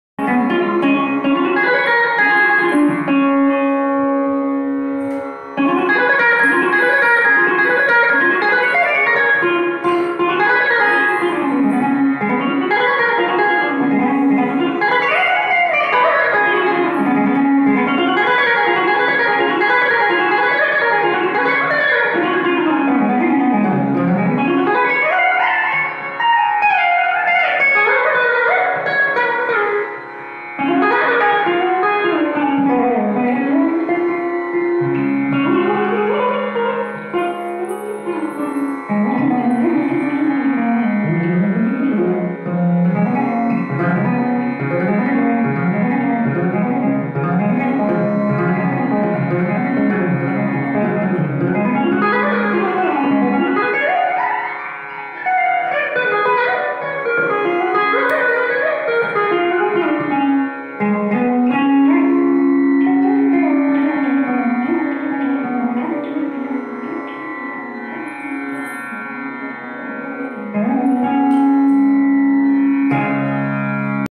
Raagam - Bindumalini || SM Subhani Mandolin
An excerpt of Raagam Bindumalini exploring on Mandolin by Sri SM Subhani...